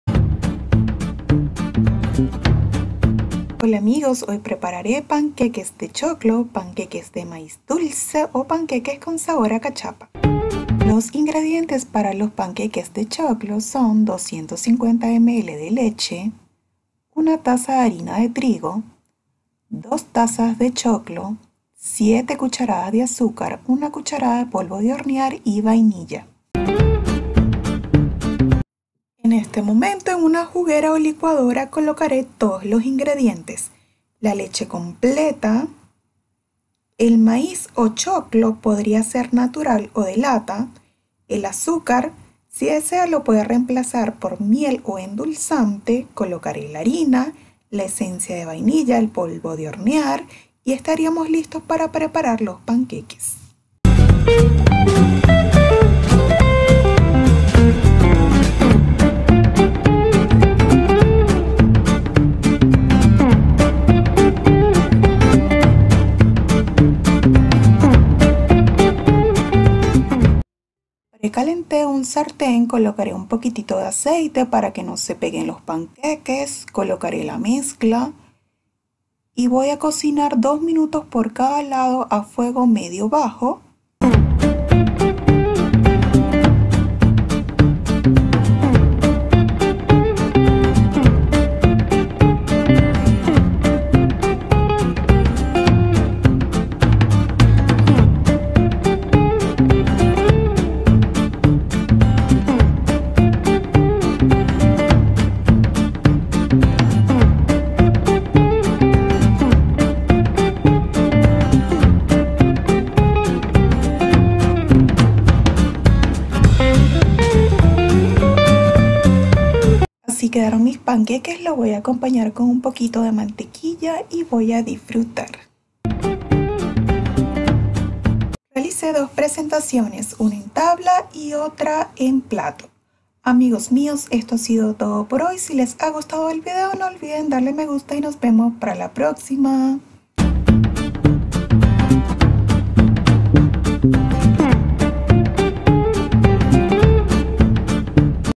Panqueques caseros de choclo /panqueques de Maíz Dulce/Panqueques con sabor a Cachapa
Deliciosos  Panqueques caseros De Choclo o maíz Dulce  Económicos fáciles y Rápidos de preparar

Ingredientes:
1 taza de harina de trigo
2 tazas de choclo/maíz Dulce
1 cucharada de polvo de hornear
7 cucharadas de Azúcar
250 ml de Leche
1 cucharada de Vainilla.